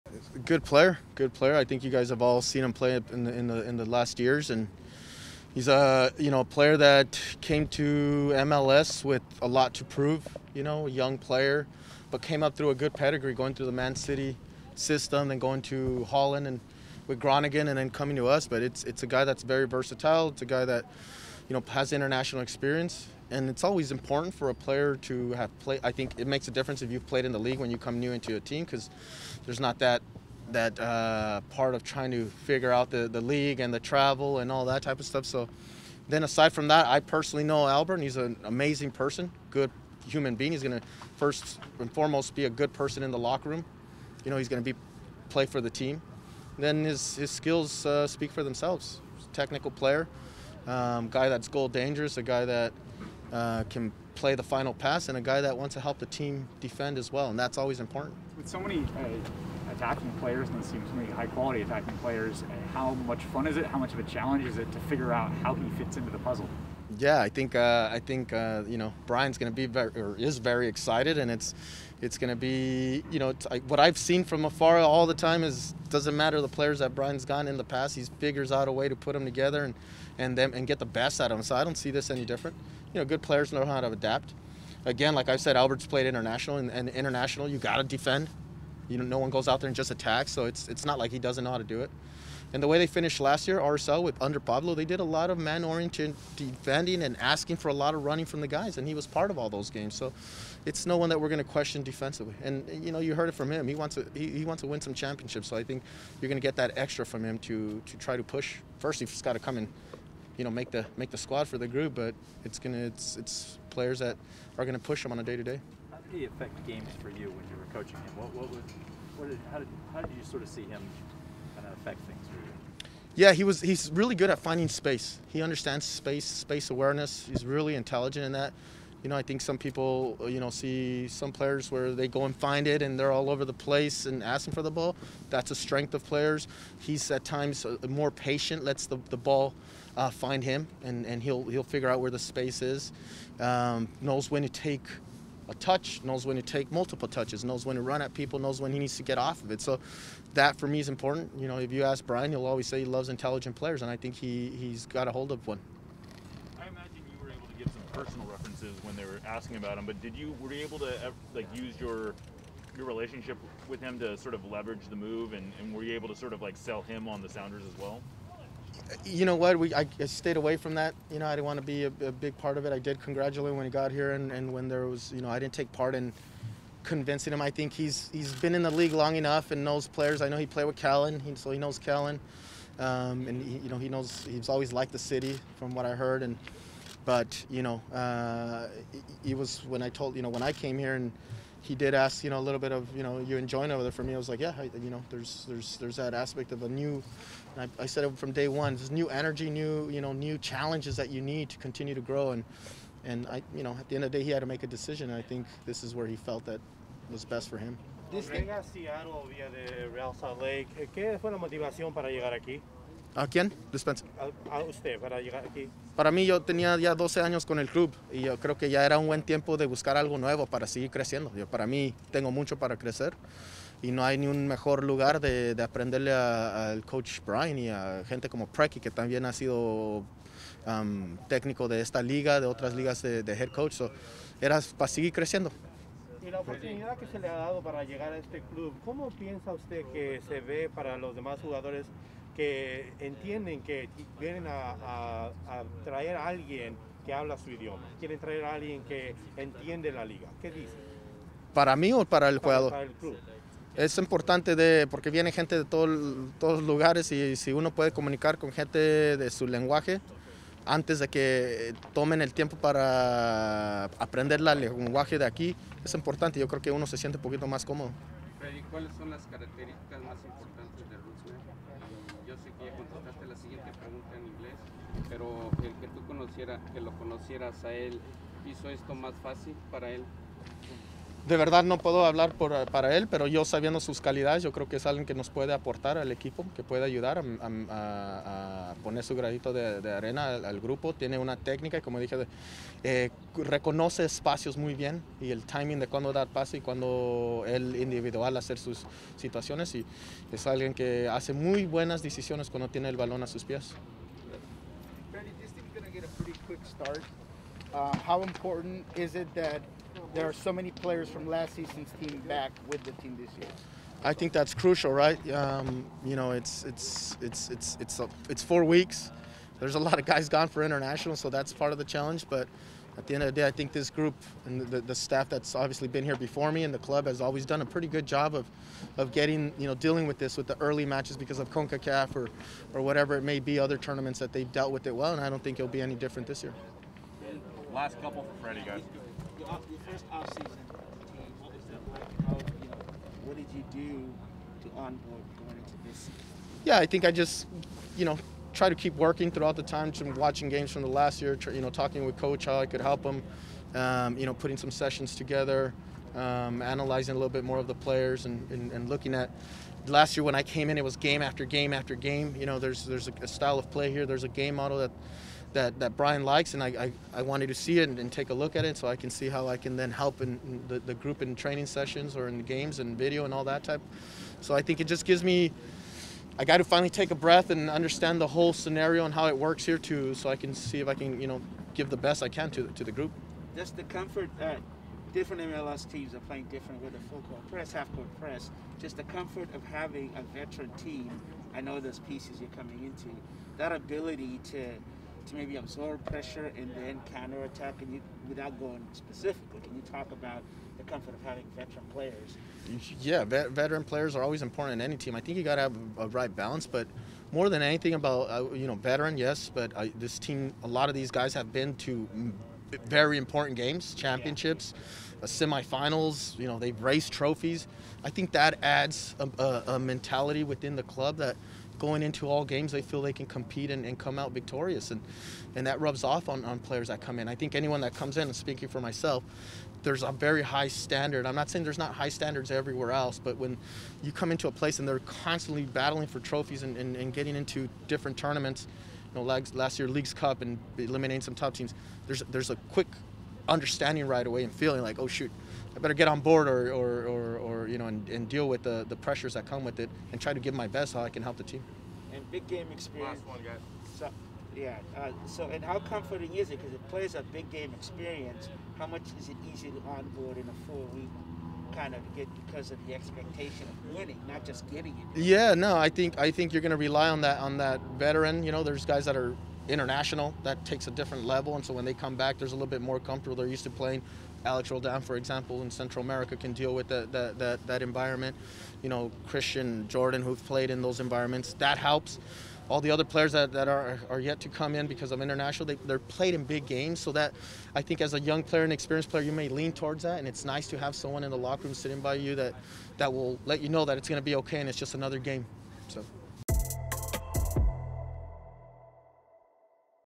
Interview: Freddy Juarez on Albert Rusnák signing with the club
Seattle Sounders FC Assistant Coach Freddy Juarez speaks to media following training on Albert Rusnák signing with the club, what the team can expect from Albert and integrating with the coaching staff.